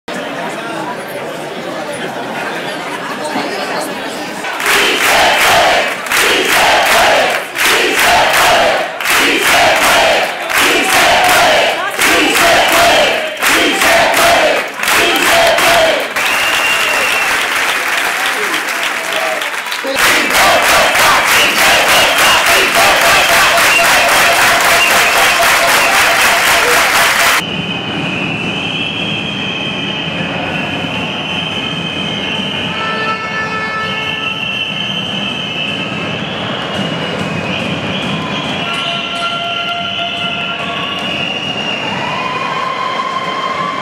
Ciudad de la imagen. TELEMADRID. Asamblea de los trabajadores
Asamblea de los trabajadores de TELEMADRID. Ya estamos organizados.